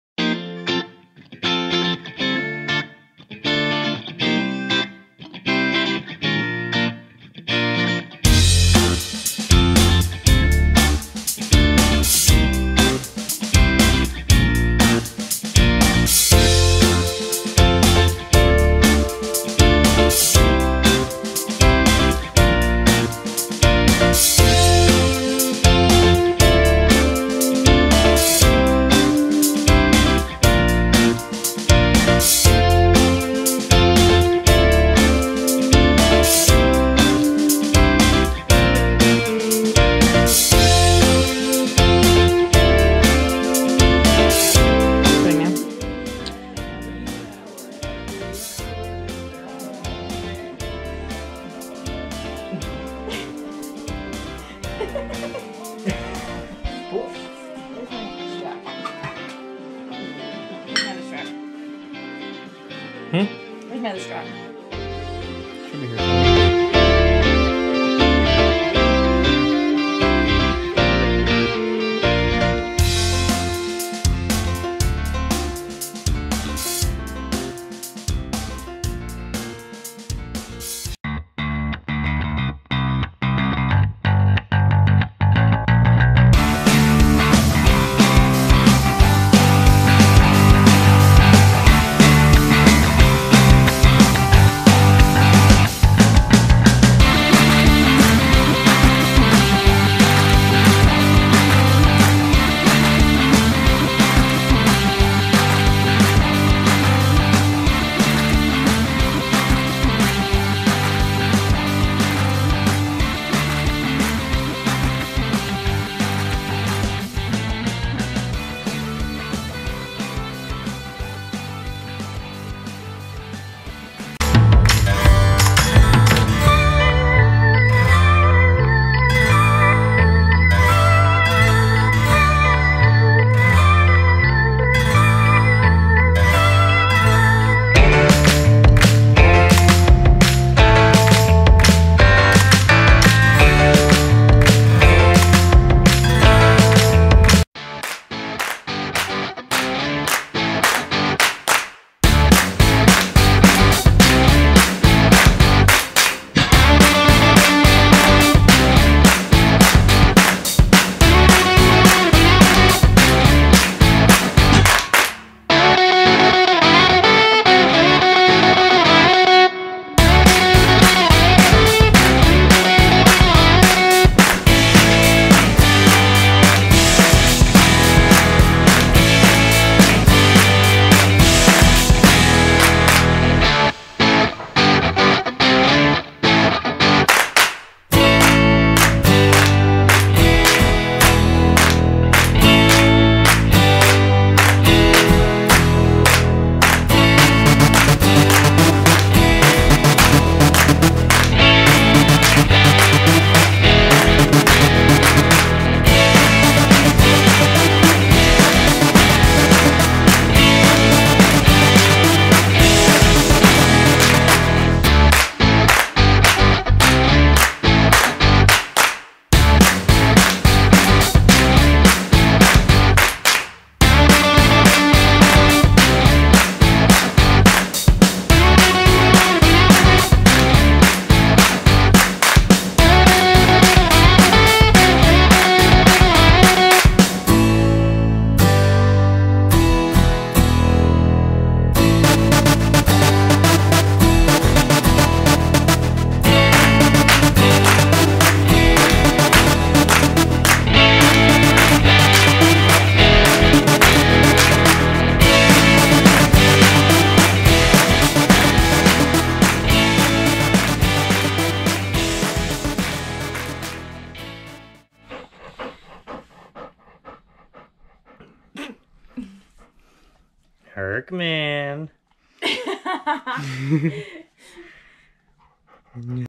Quarantine Back Day
Welcome to our Quarantine back day featuring Tac the bearded dragon and Herc the red fox lab. Lets lift folks!